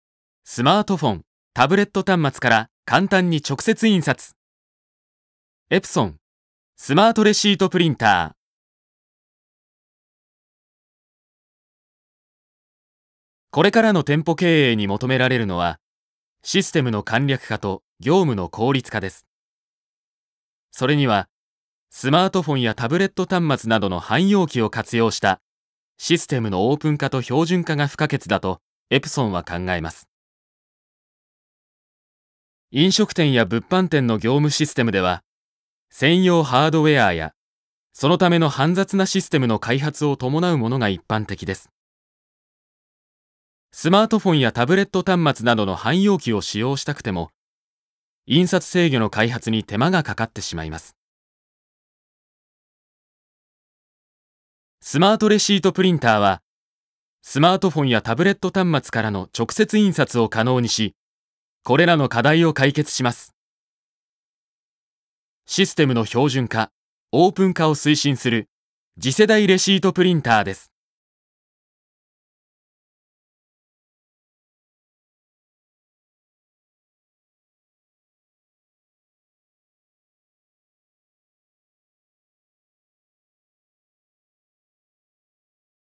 エプソン レシートプリンター TM-T88V-i、TM-T70-i紹介動画①
スマートフォンやタブレットからの直接印刷が可能なTM-T88Ⅴ-i、TM-T70-iの商品コンセプトをご紹介します。